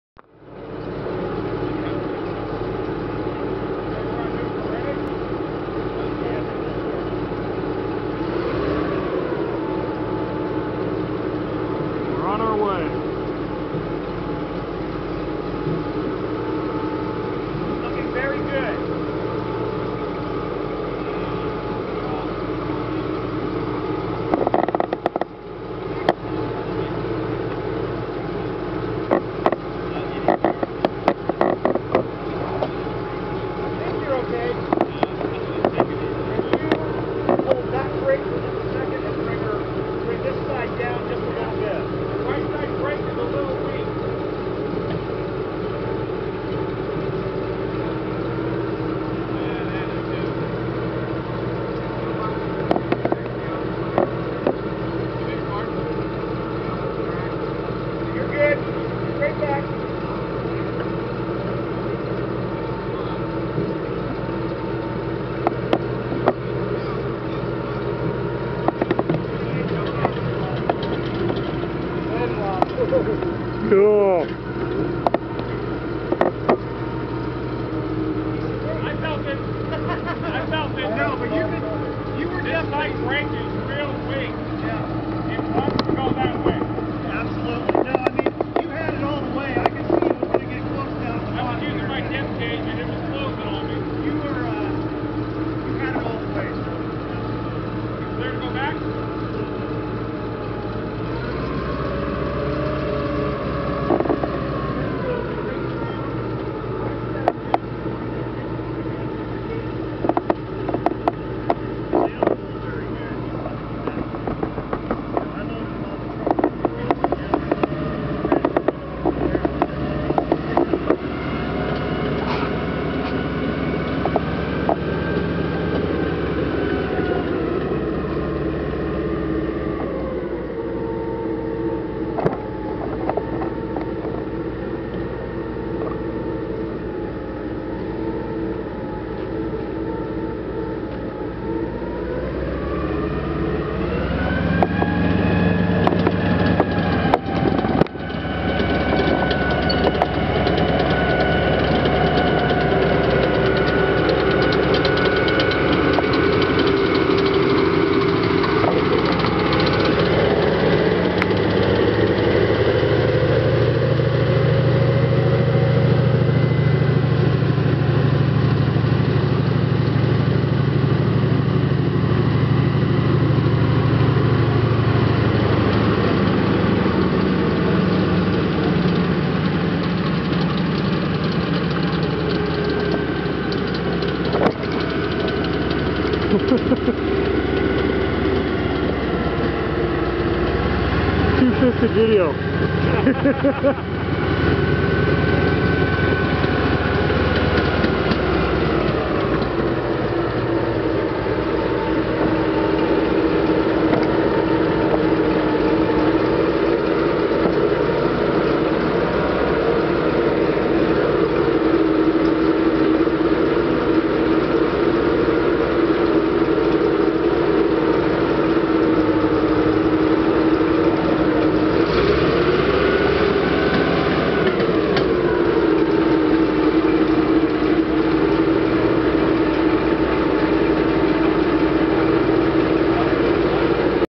M39 AUV at the MVTF
The Military Vehicle Technology Foundation received a M39 AUV on Saturday, 10SEP2011.  It was donated to the Foundation by the Upton brothers of Calistoga.